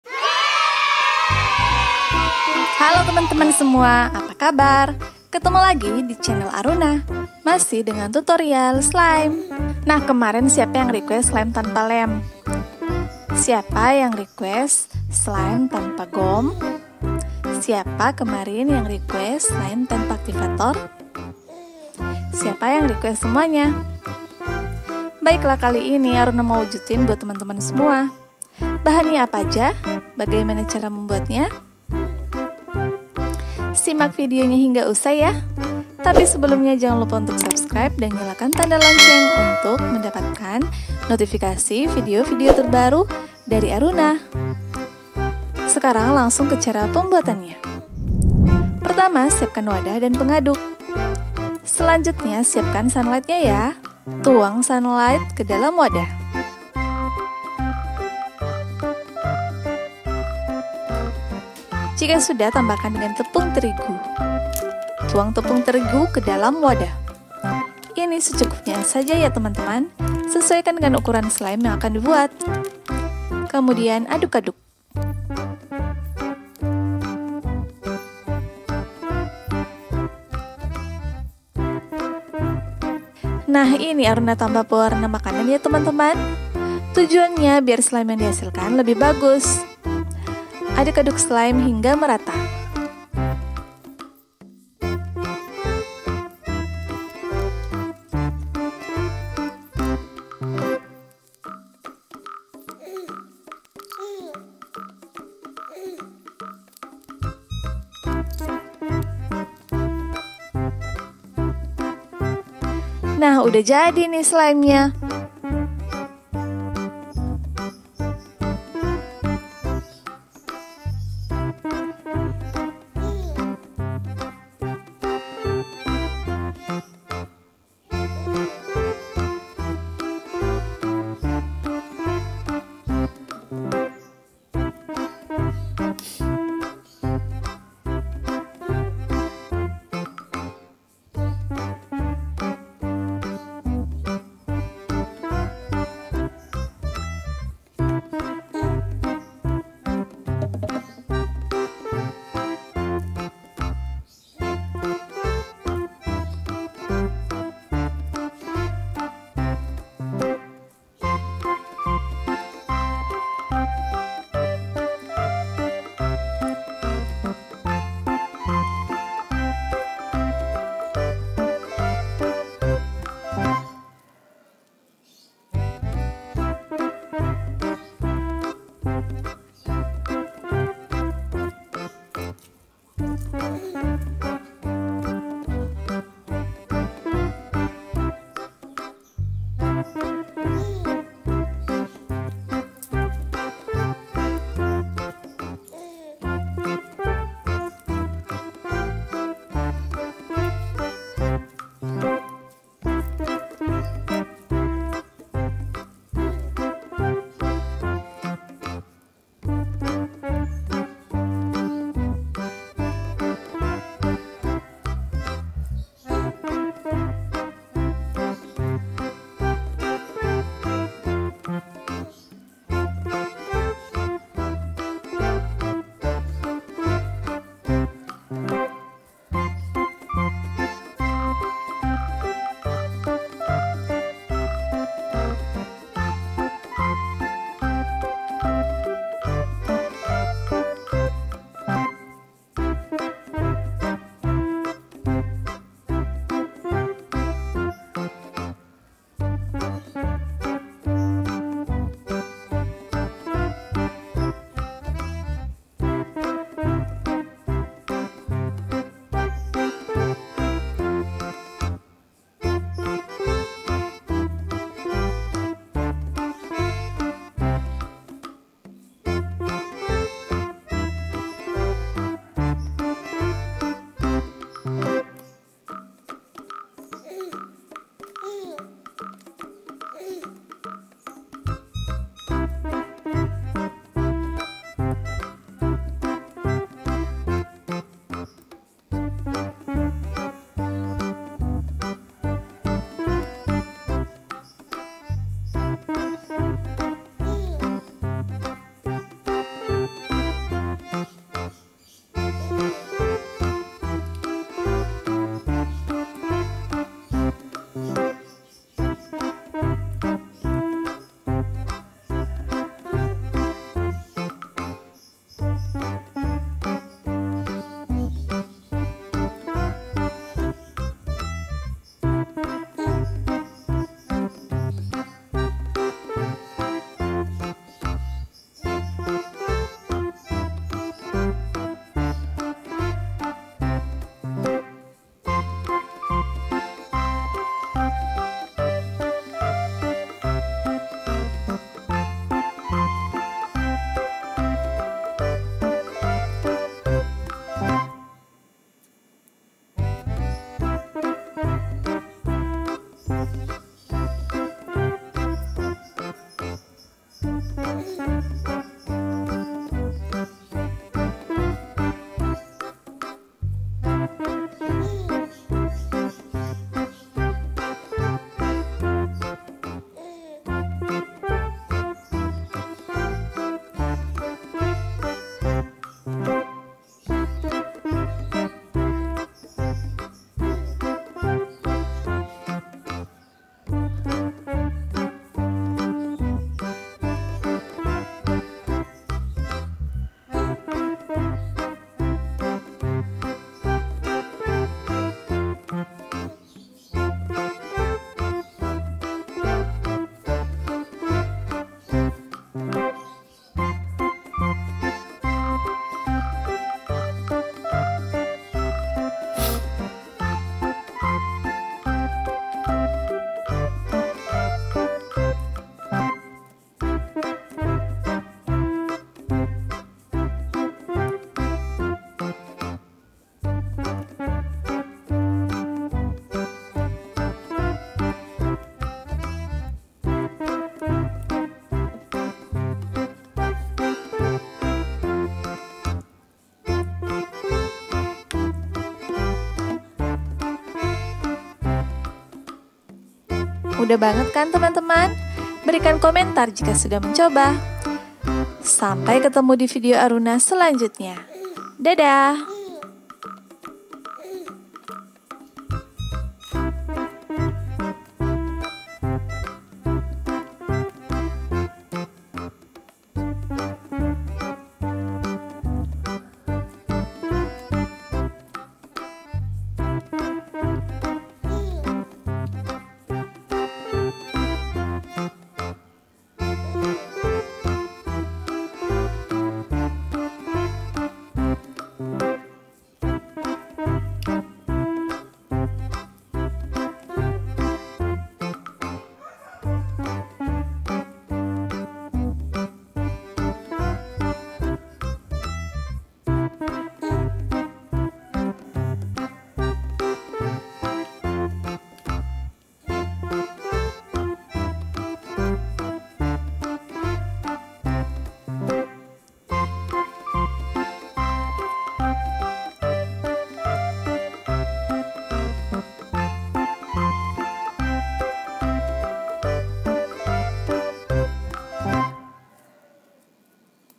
Cara Membuat Slime Dari Tepung
Hallo teman-teman semua, kali ini Aruna mau share Cara Membuat Slime Dari Tepung

Bagaimana cara membuatnya??? Simak terus videonya hingga usai yah.

Simak juga video video Aruna lain tentang, cara membuat slime, cara bikin slime, dan cara membuat slime lainnya.